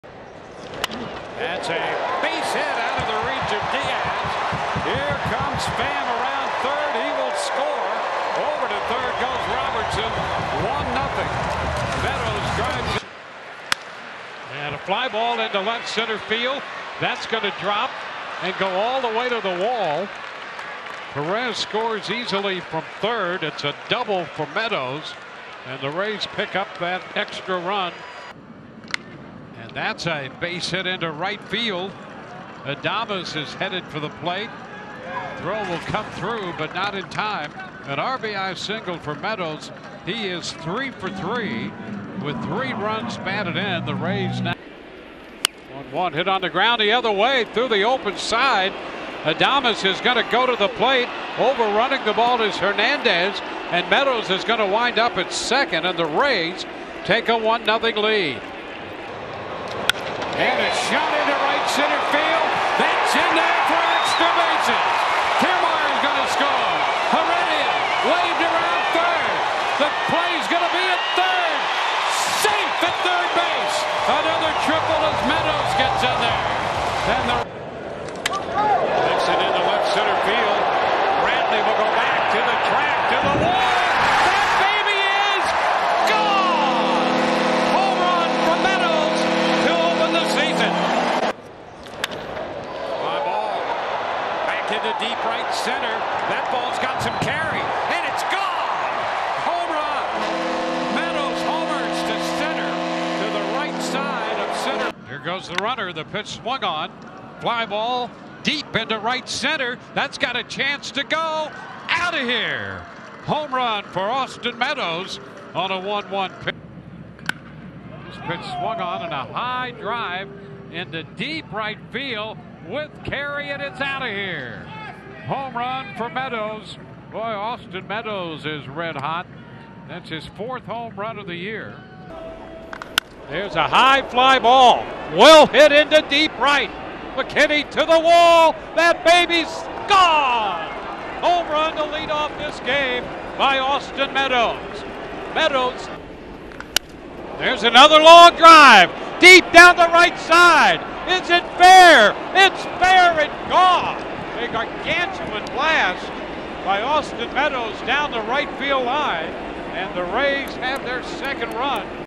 Austin Meadows 2019 March/April Highlights
2019 March/April Stats - .351 .422 .676 1.097, 20 G, 83 PA, 26 Hits, 6 HR, 19 RBI, 8 BB to 18 SO, 1 Player of the Week Award